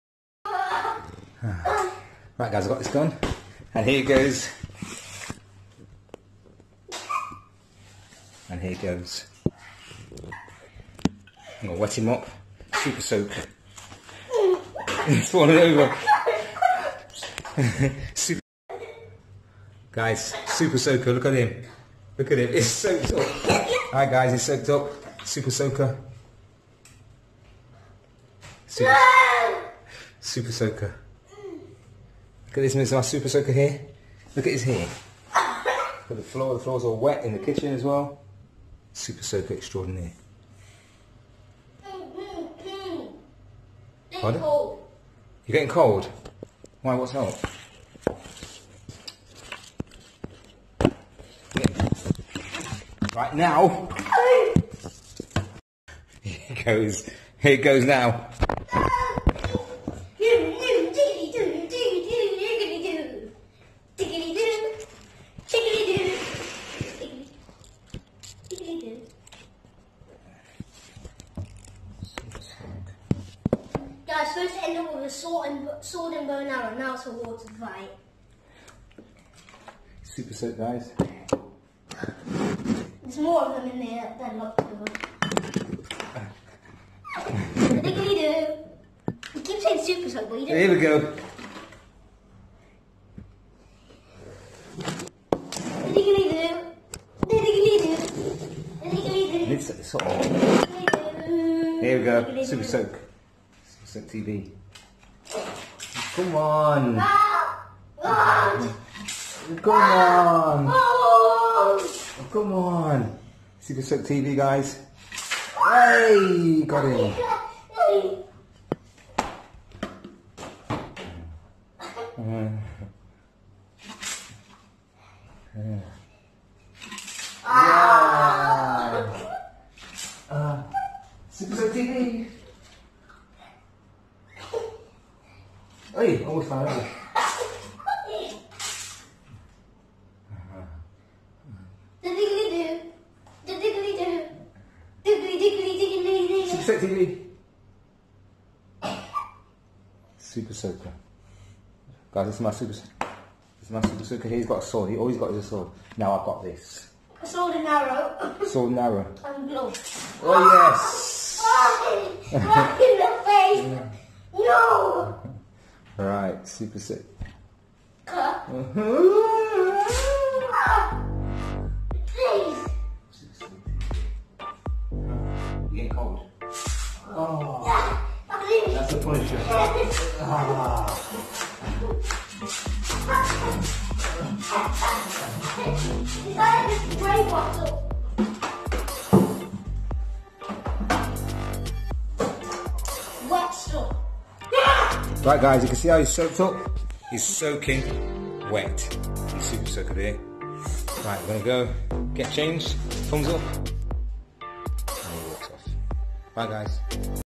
lad gets soaked
Lad gets absolutely soaked by dad after trying to battle dad with a sword